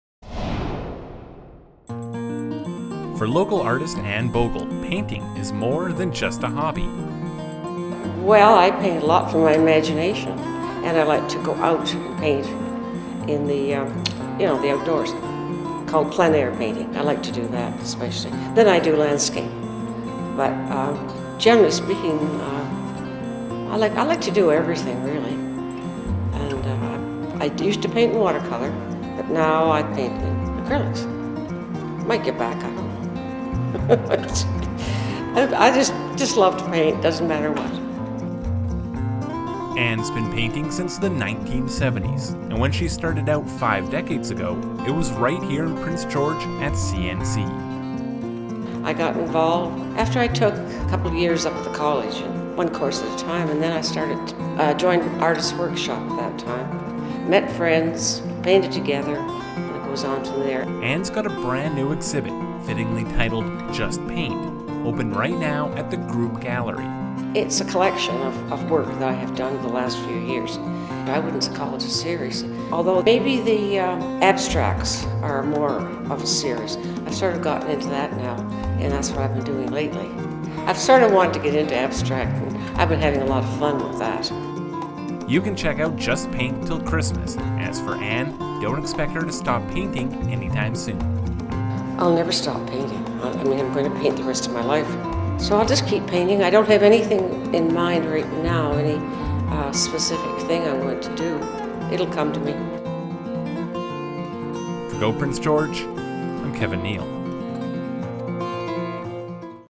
Just Paint
Prince George artist Anne Bogle has a new exhibit.